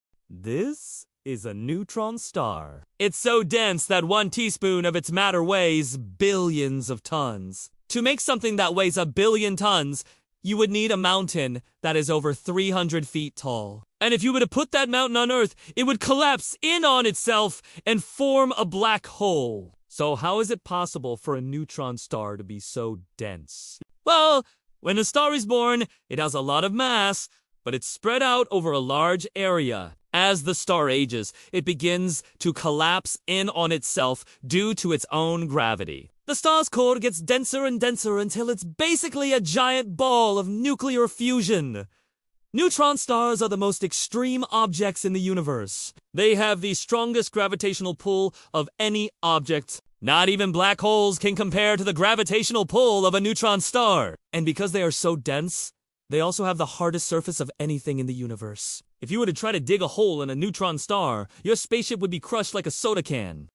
🌟 Neutron Stars Explained in 60s | Stronger Than Black Holes?
A neutron star is so dense that one teaspoon weighs billions of tons. 🌌 Learn why these cosmic monsters are among the most extreme objects in the universe.